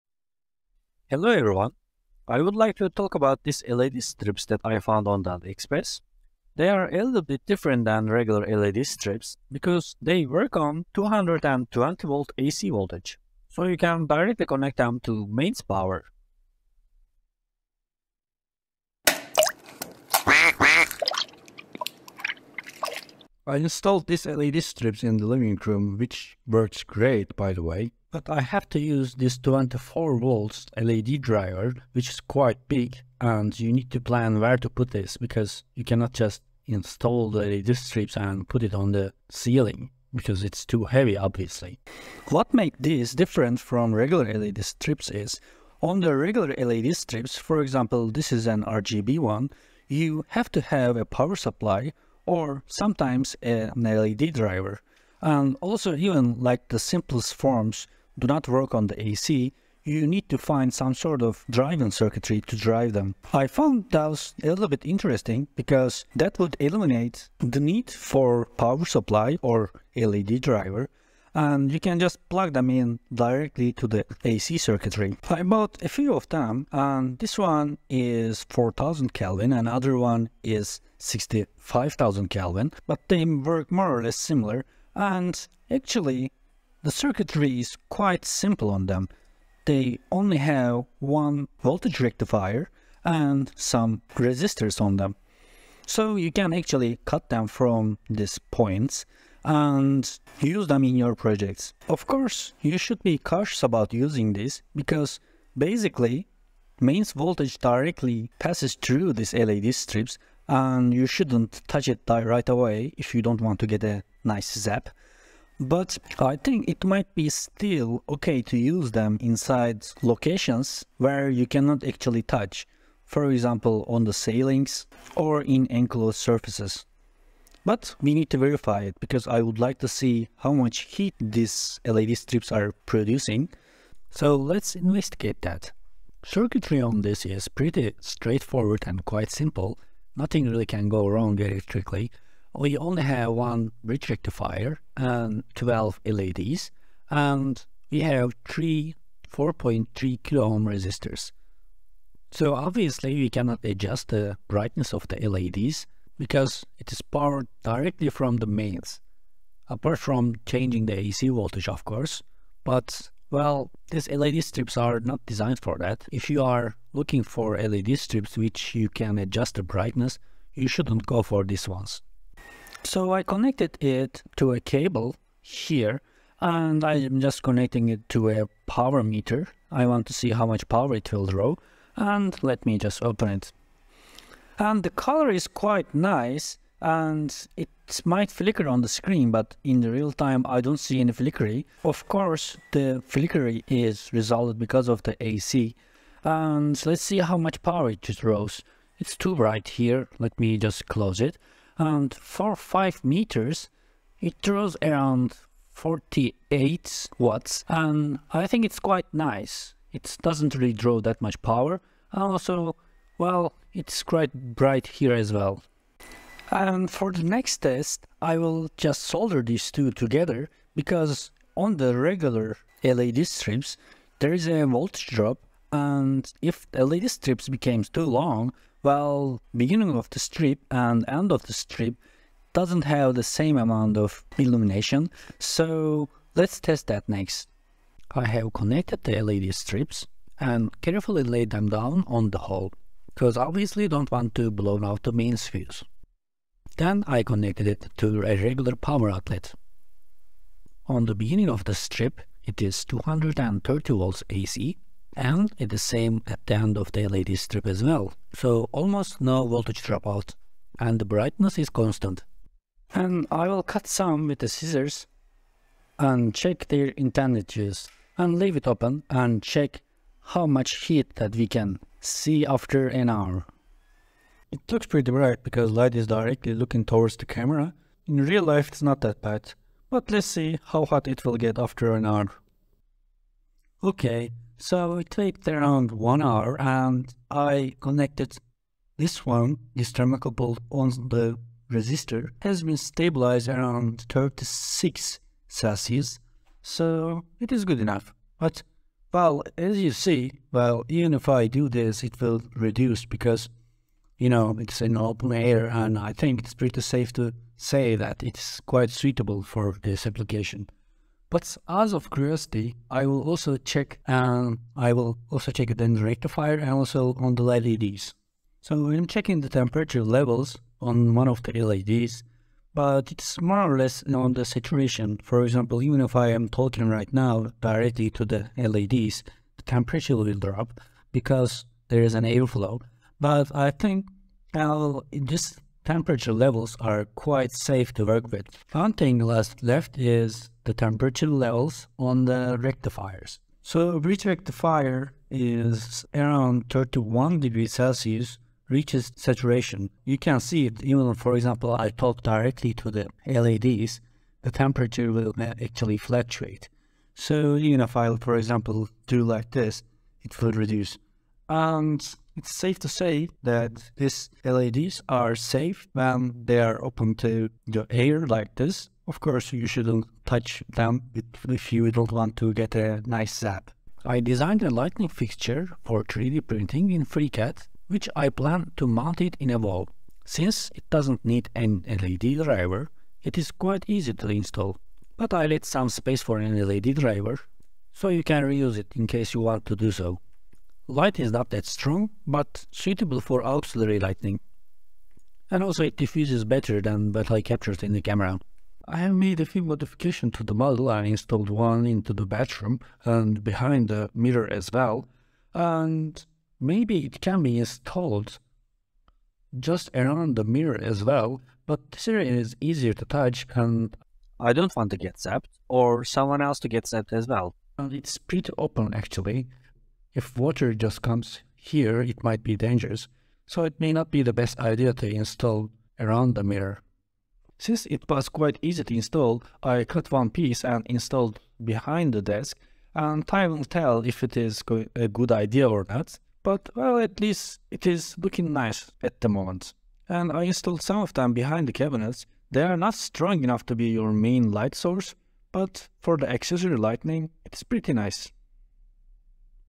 220/110V "AC" LED Strips at home
Say goodbye to power supplies and LED drivers and hello to the power of 220V AC LED strips!

In this video, we thoroughly test and  220V AC LED strips, evaluating their brightness, heat, and usability. We provide all the information you need to make the right choice for your specific needs and space, whether you're lighting up your home, office, or outdoor area. From easy installation to stunning results, these LED strips pack a powerful punch that will enhance any environment. So join us as we explos we explore the world of 220V AC LED strips and take your lighting game to the next level!